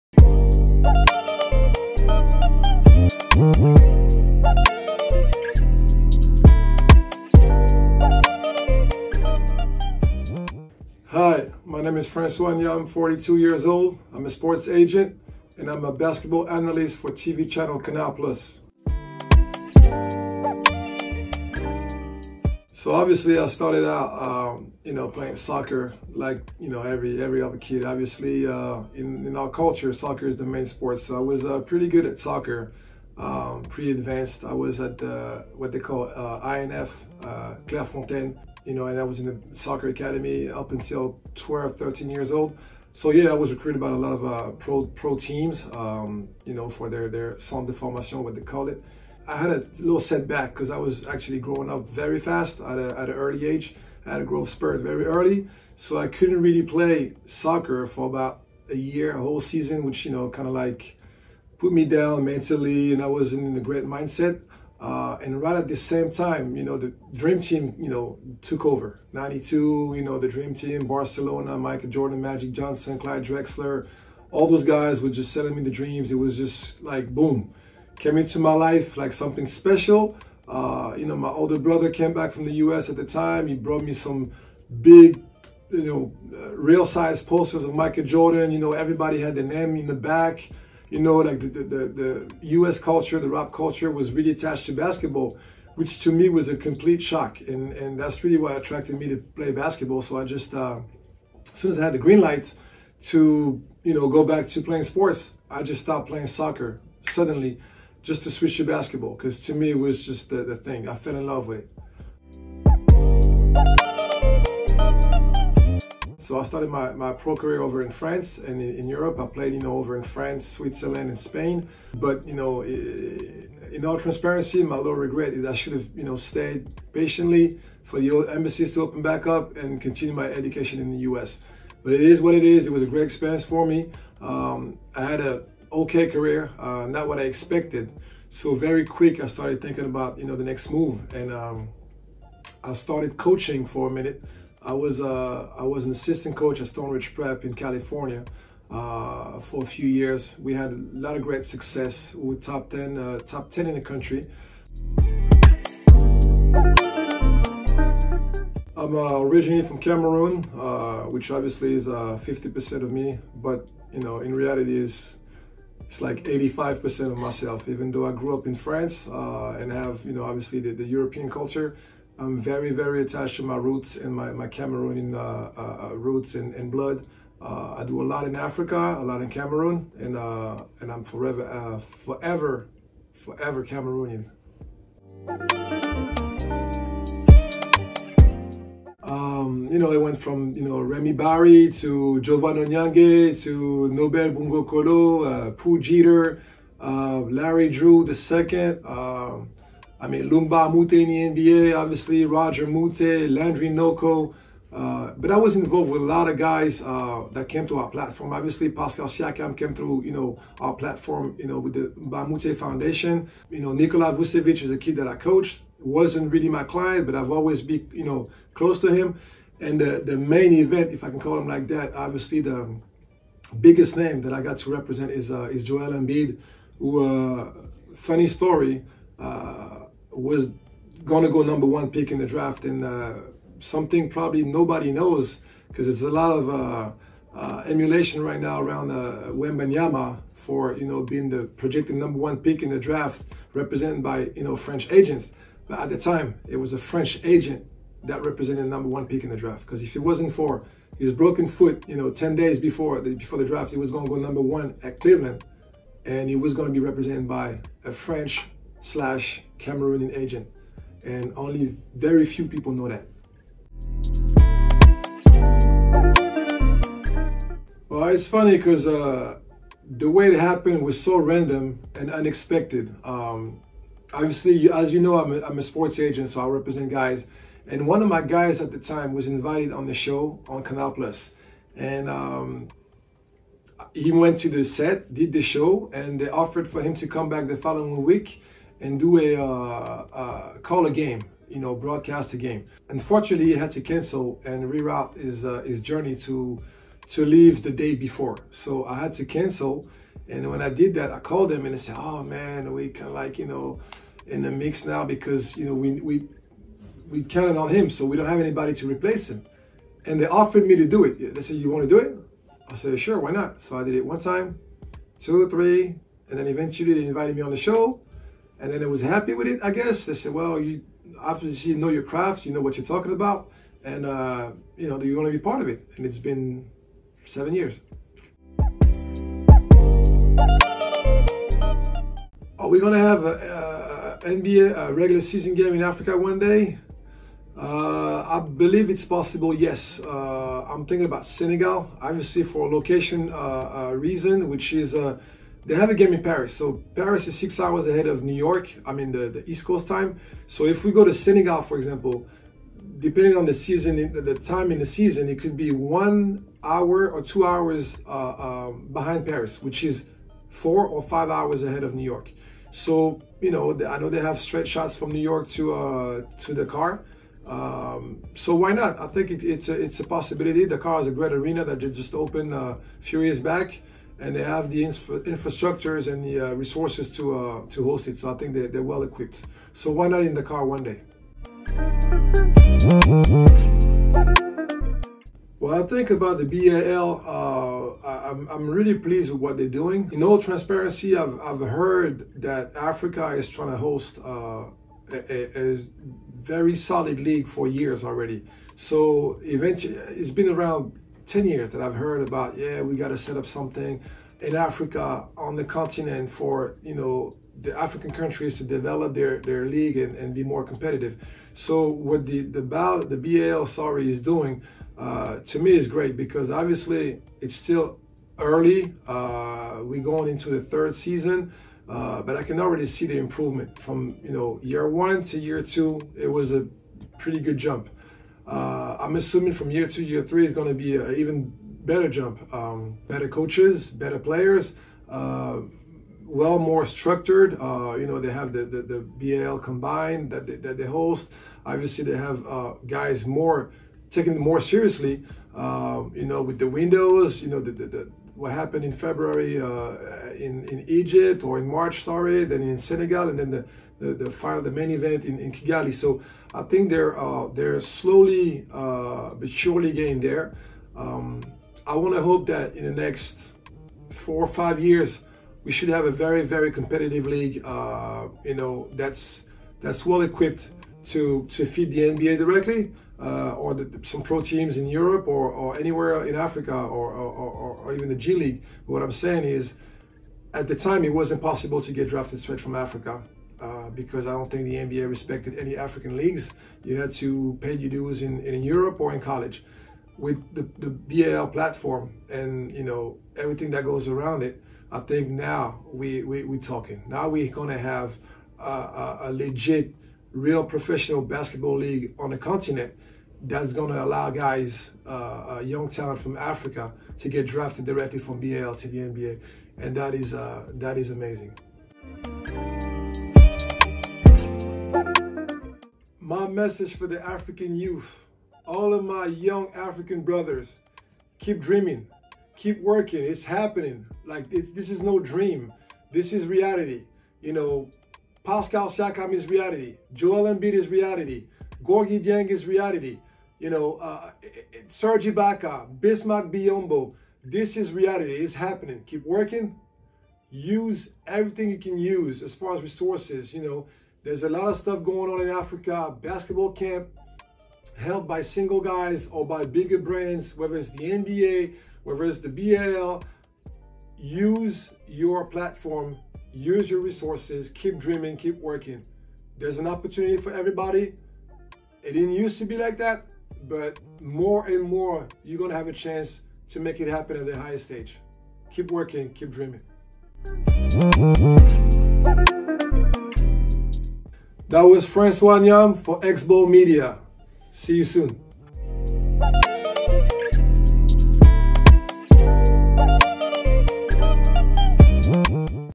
ALL EYEZ ON FRANÇOIS NYAM! (VOSTFR) His career, Joel Embiid, Canal Plus, the BAL ...
François Nyam was our last guest in ALL EYEZ ON ME! Find out who's the Cameroonian agent/consultant...
Give us your feedbacks in comments XBALL FAMILY!